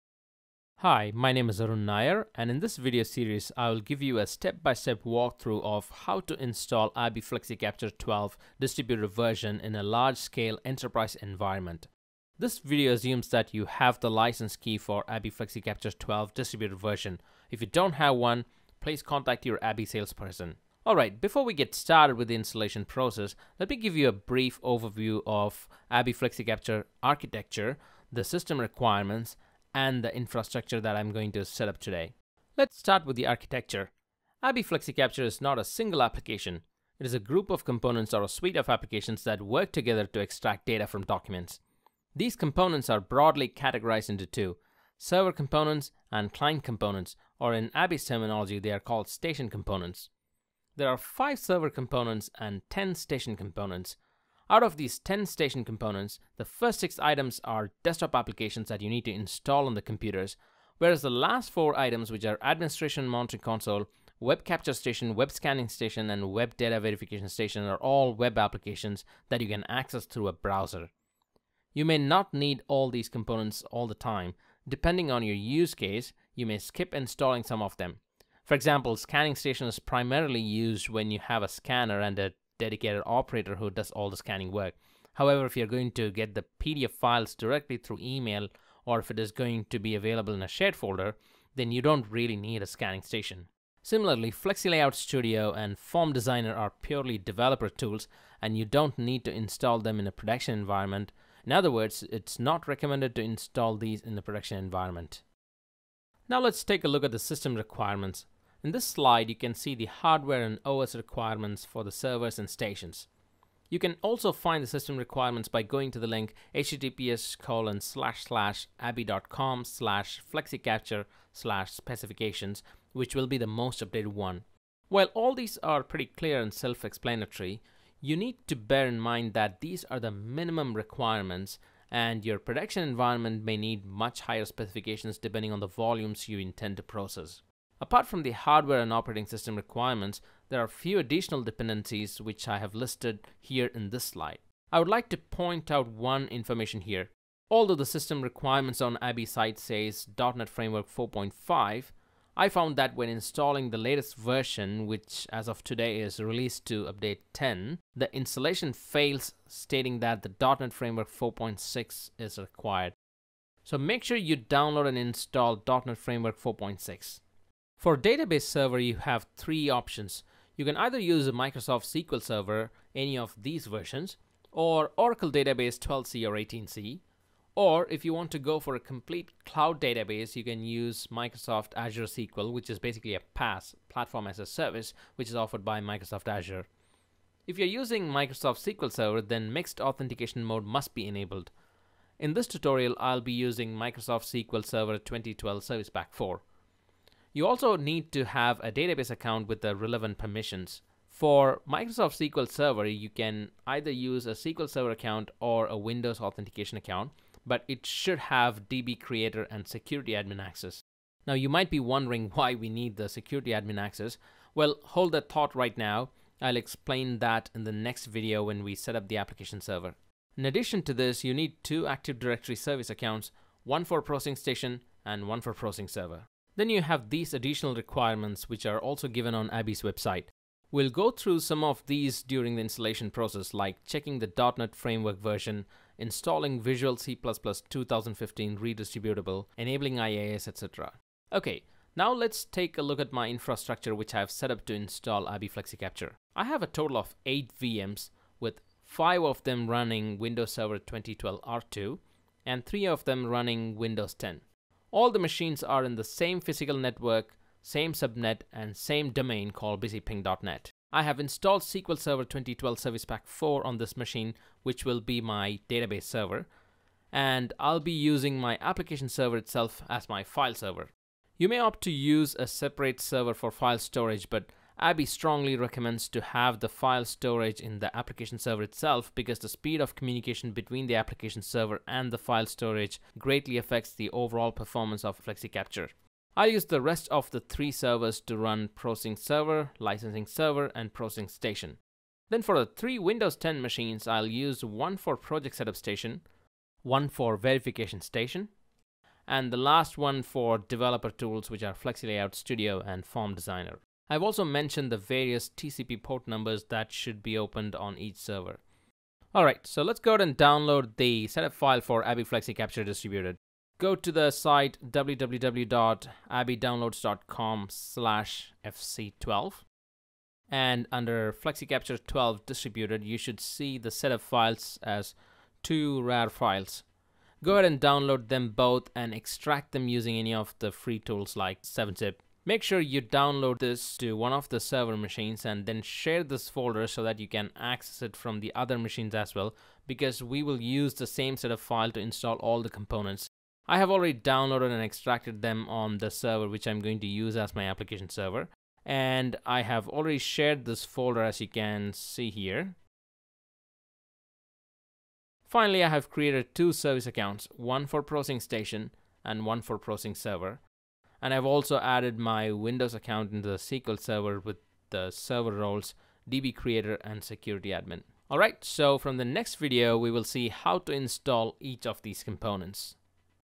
ABBYY Flexicapture Distributed Installation | Part 1 | Introduction
This tutorial series will give you step by step walk through of how to install ABBYY Flexicapture 12 Distributed installation in an enterprise environment.

In this video we will discuss about the ABBYY Flexicatpure 12 Distributed Architecture